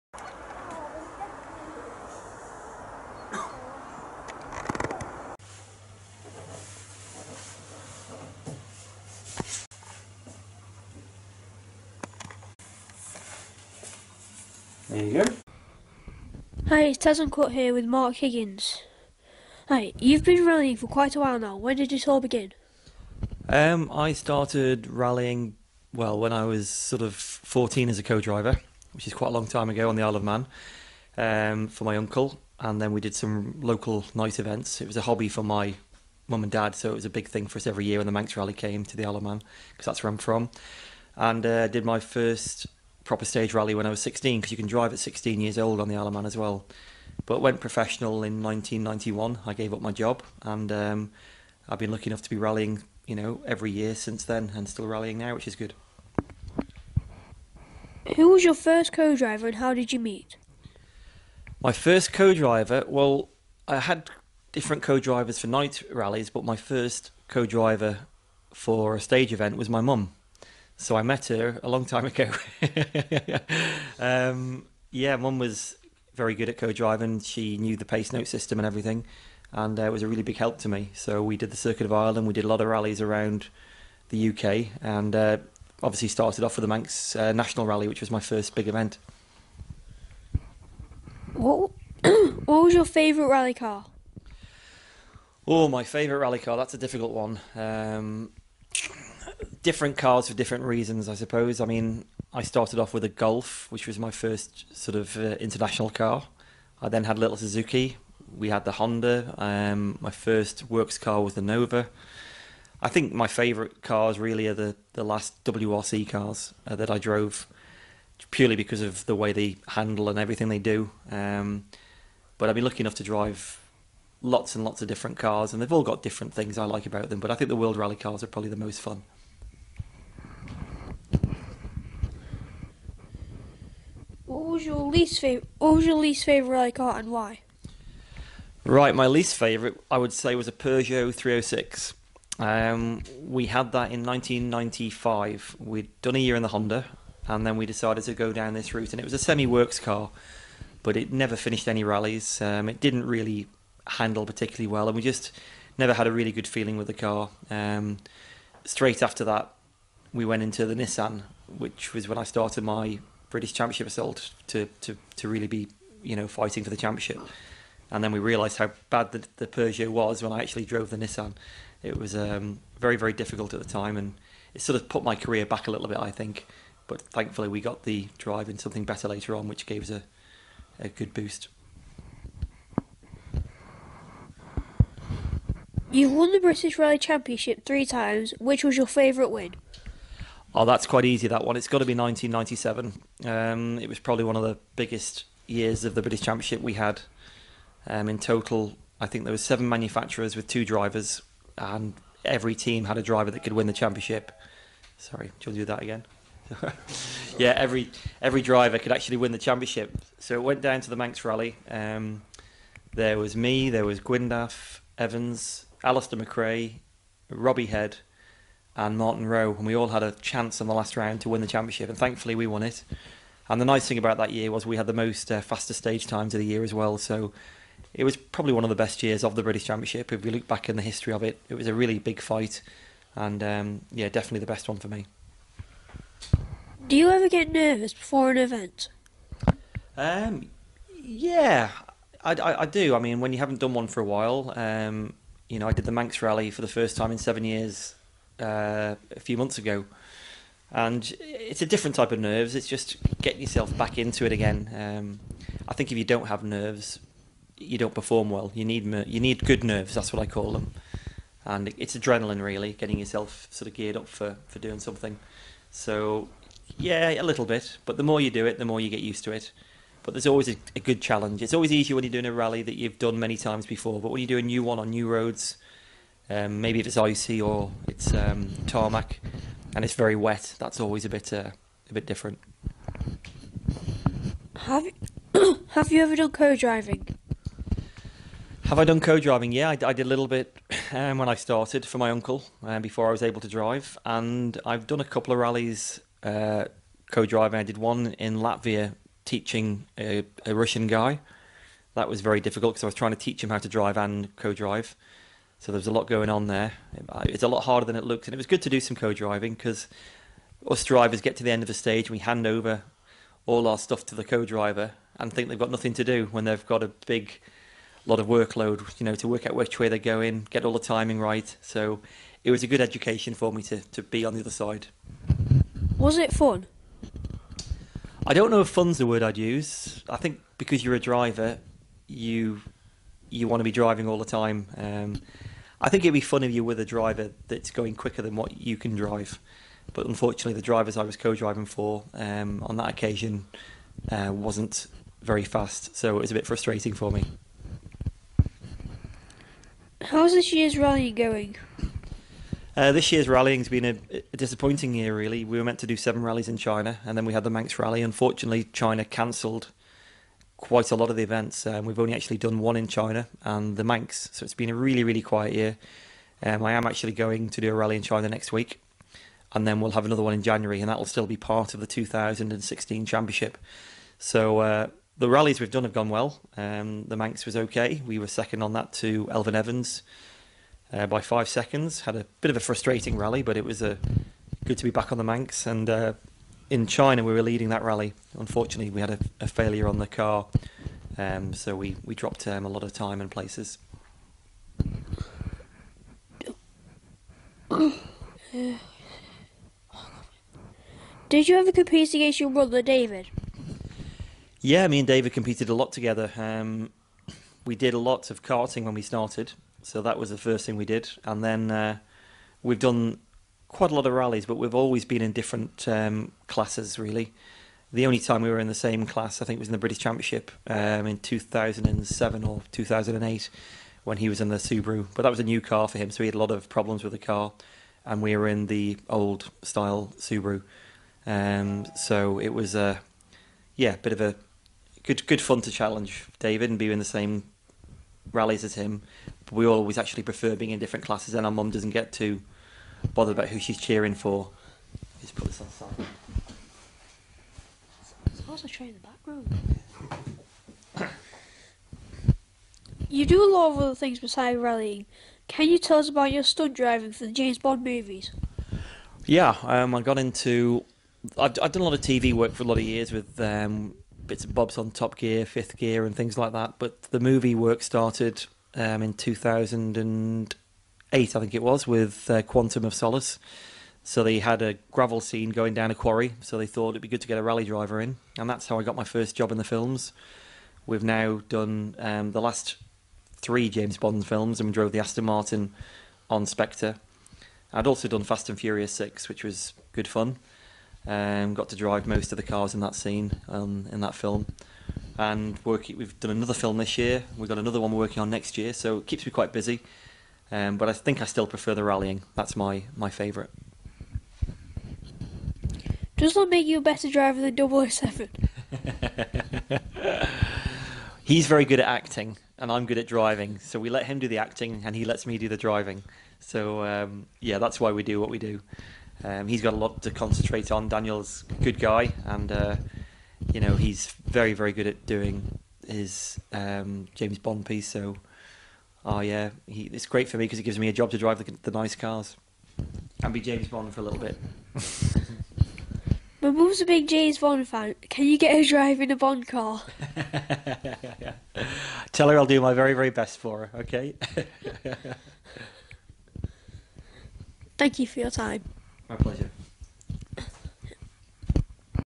Mark Higgins Interview
This is my interview with triple British Rally Champion Mark Higgins.
Mark is also a stunt driver and has worked on the last three Bond movies and Fast and the Furious 6.